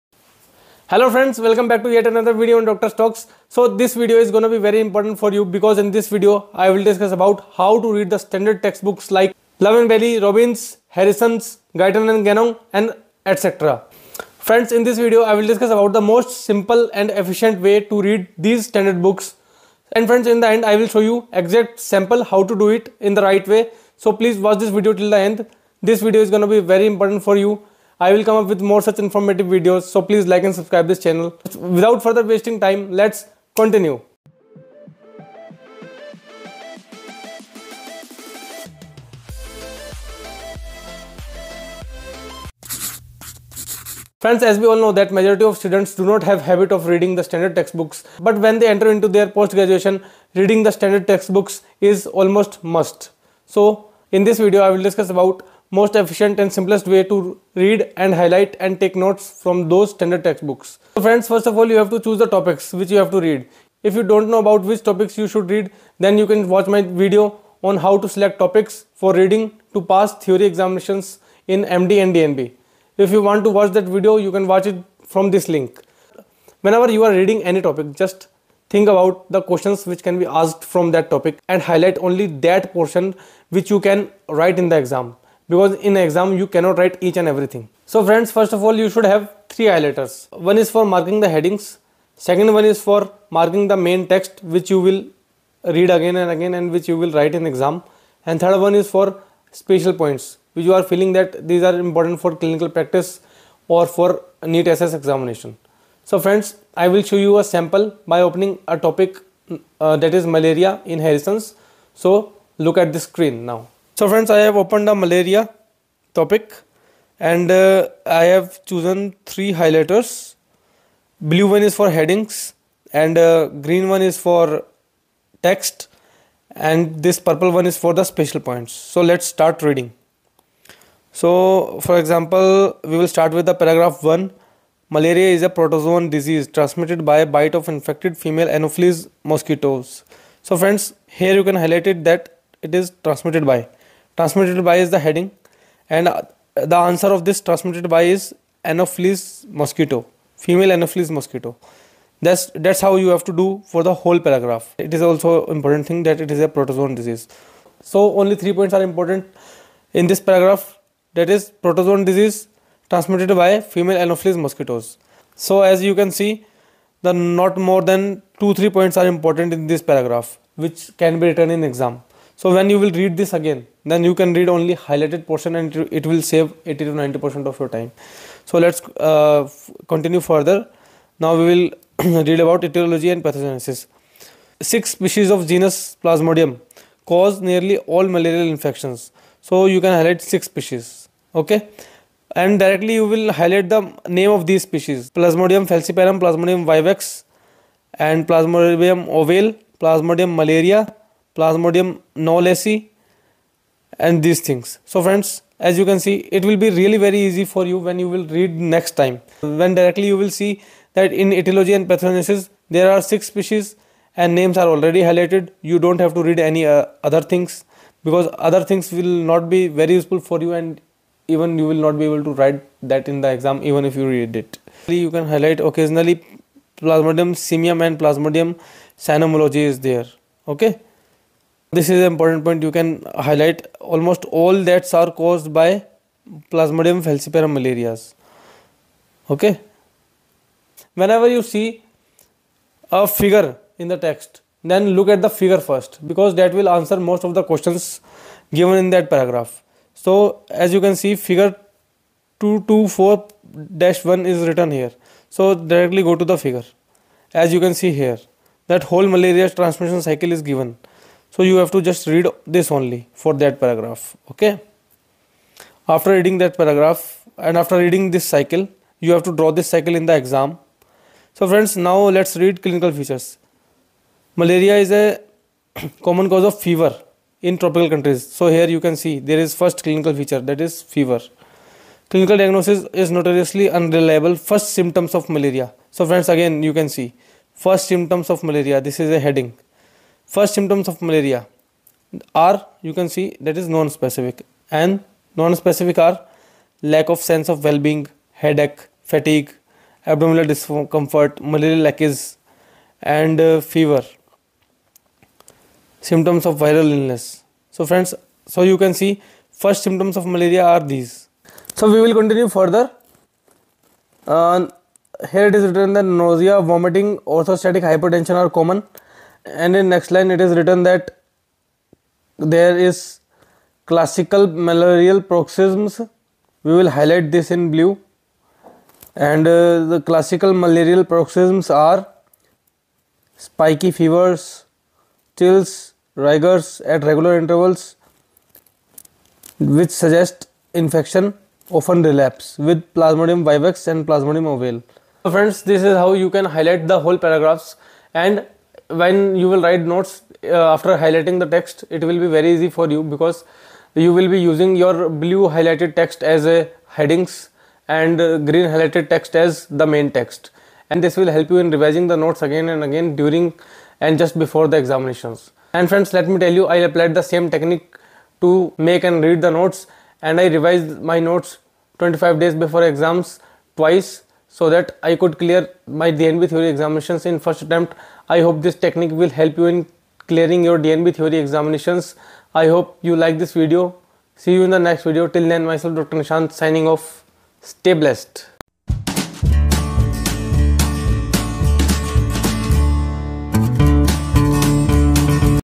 How to read Harrison internal medicine | How to study bailey and love surgery | Best way to study
How to read Harrison internal medicine | How to study bailey and love surgery | Best way to study internal medicine

In this video I have tried to explain the technique which I used during my post graduate and MBBS times for reading the books .
I hopw this video will help you in reading better and highlihting the relevant words only which are relevant for your theory and practical exams